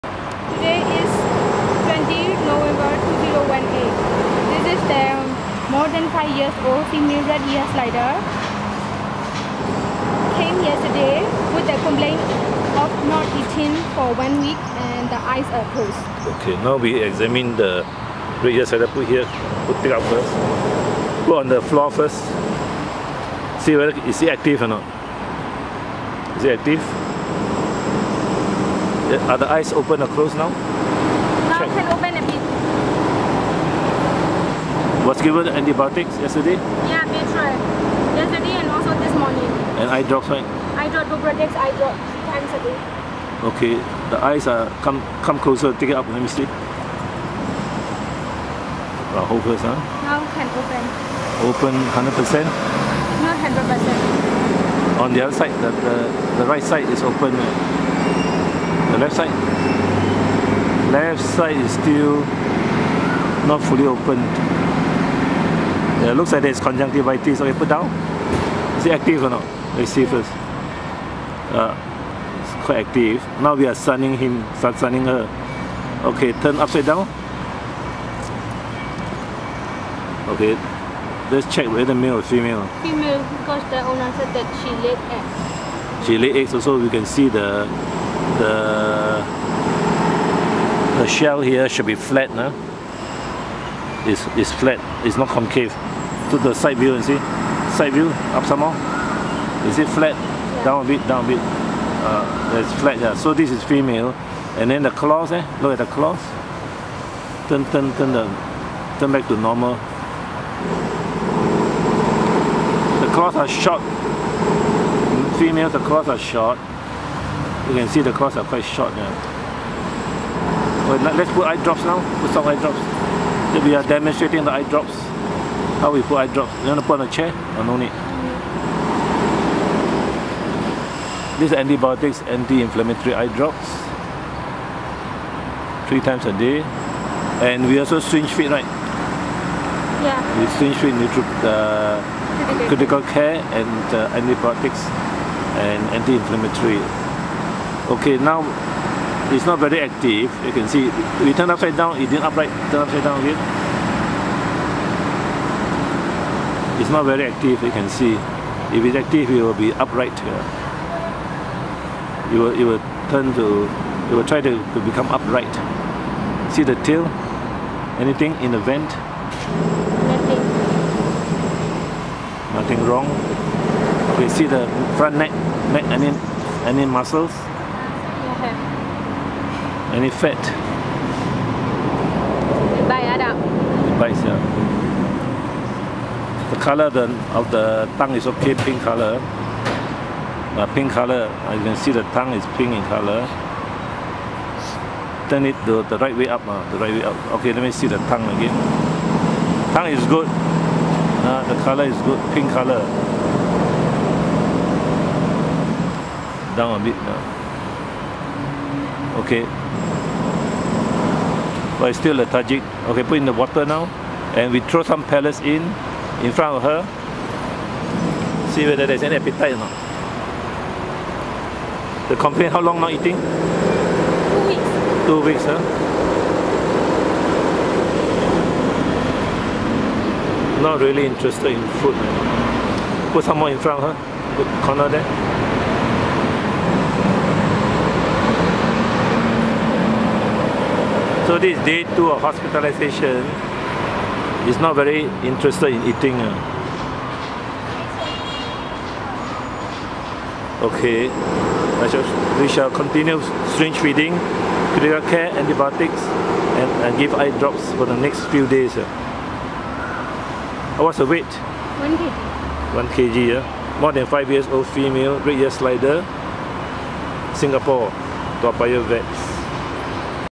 Day 2. A 5-year-old female red-eared slider has closed swollen eyes and no appetite
Day 2.  Nov 20, 2018.  Inpatient.
The lethargic slider is treated in-patient with antibiotics, syringe feeding of Critical Care and sunlight. Still not eating today and not active.

Update:  It took more than a week to recover.